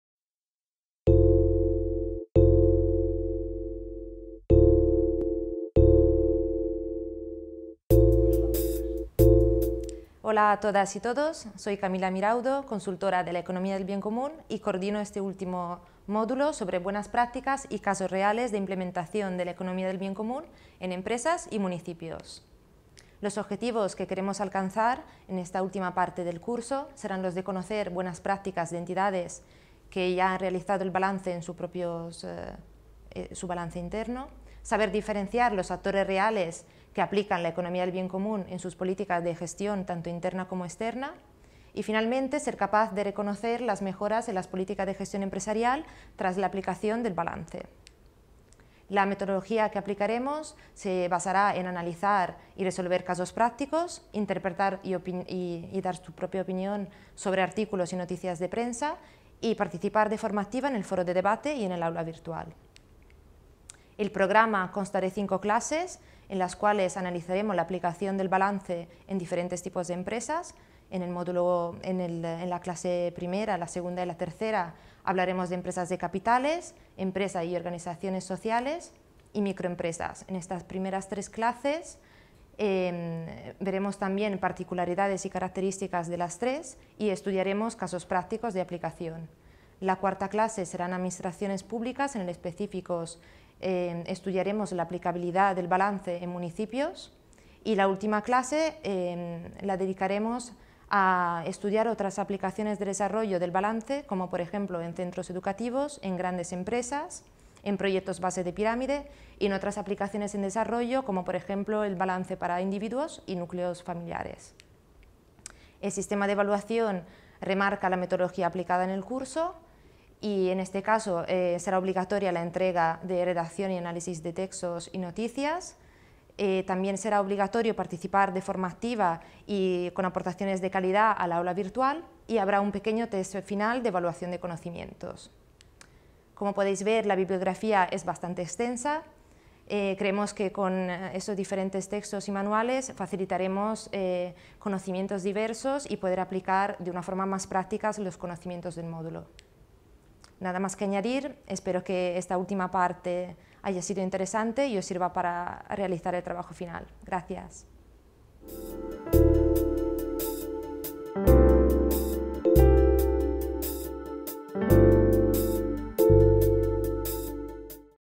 Módulo 9 del Diploma de Especialización Profesional en Consultoria de la EBC
Autor: Miraudo, Camilla
Serie: PIE Economía del Bien Común
Año: 2018
Resumen: Esta asignatura tiene como objetivo principal facilitar al alumnado acceso a casos reales de aplicación de la Matriz y del Balance del Bien Común en empresas y organizaciones, así como en la administración pública, en concreto en municipios. A lo largo del curso hemos podido conocer los principios de la Economía del Bien Común, sus herramientas de aplicación y de verificación. En esta recta final, podemos acercarnos más a casos reales y finalmente analizar en detalle la aplicabilidad del BBC a entidades reales de nuestra sociedad. Analizaremos también, las buenas prácticas y las mejoras que se han, y se están poniendo en práctica en los diferentes actores analizados. Sin embargo, no hablaremos exclusivamente de empresas consideradas clásicas, sino que intentaremos detallar más específicamente las diferentes categorías de empresas y organizaciones, las Administraciones Publicas, y su grado de implicación, y otros actores de la sociedad como organizaciones no gubernamentales y asociaciones.